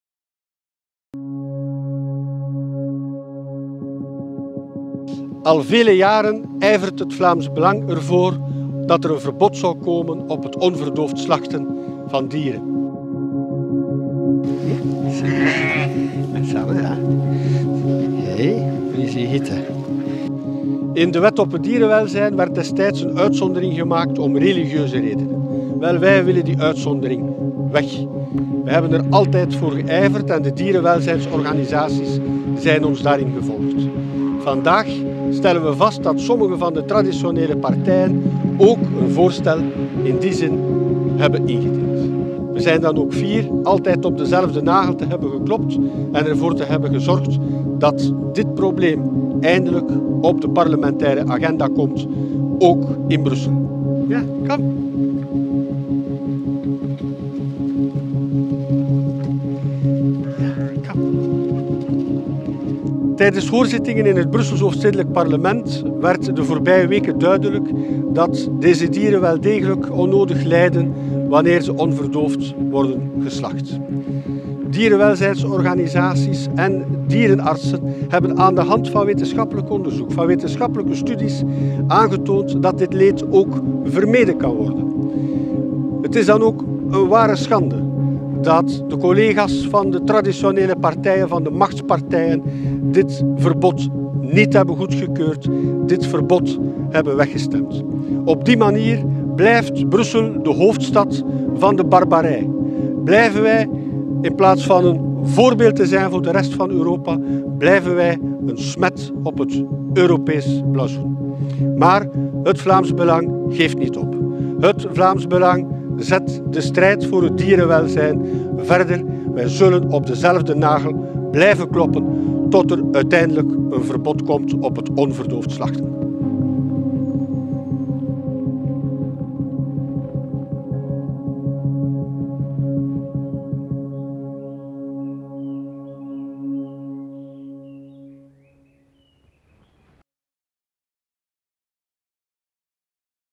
Wij zetten onze strijd verder!
Het Brussels Hoofdstedelijk Parlement stemde vandaag TEGEN een verbod op onverdoofd slachten. Dit tegen alle adviezen van dierenrechtenorganisaties én wetenschappers in. Het is duidelijk dat de systeempartijen niet wakker liggen van het onnodig lijden van deze dieren. Het Vlaams Belang geeft echter niet op en zet de strijd voor een verbod op onverdoofd slachten verder!